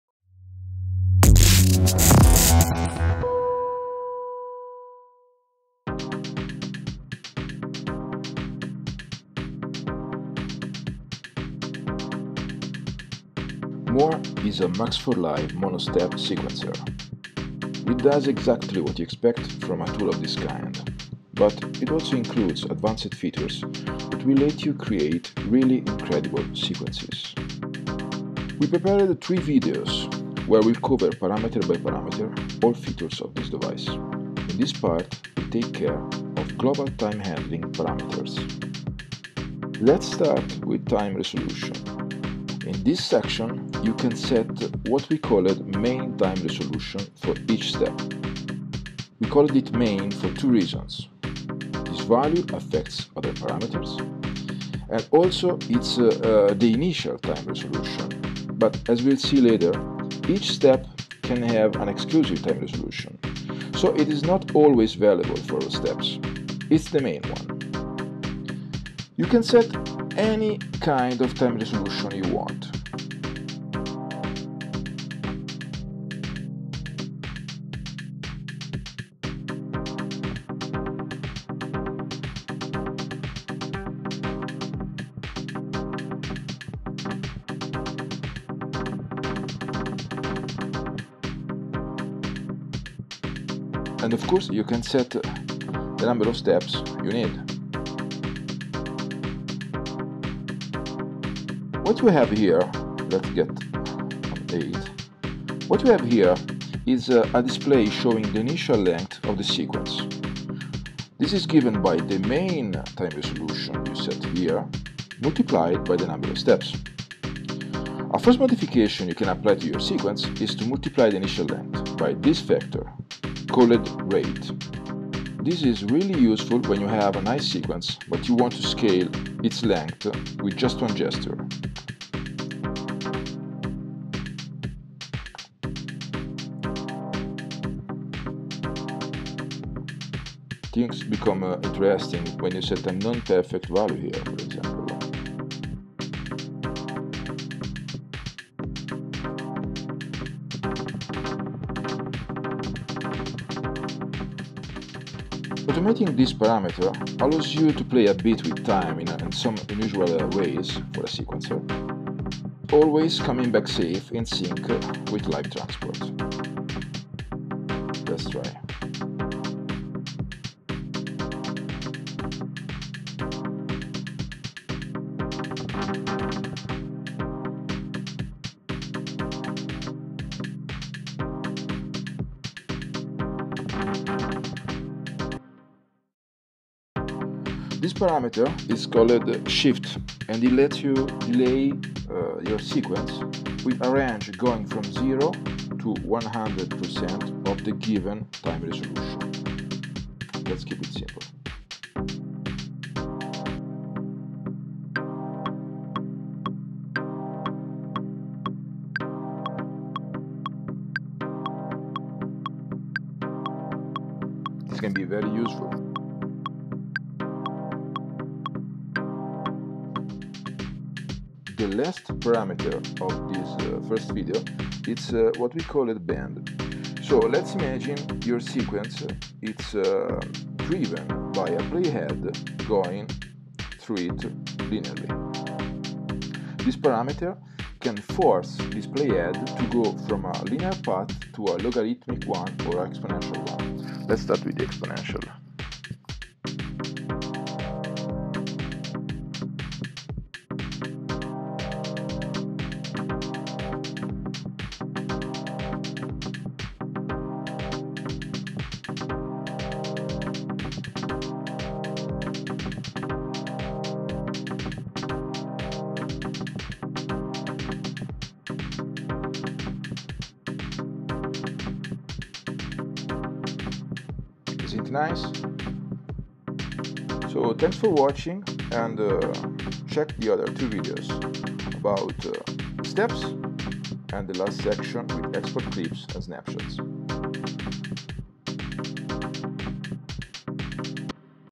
MOOR showcase - part 1/3
MOOR is a Max For Live mono sequencer with unique features.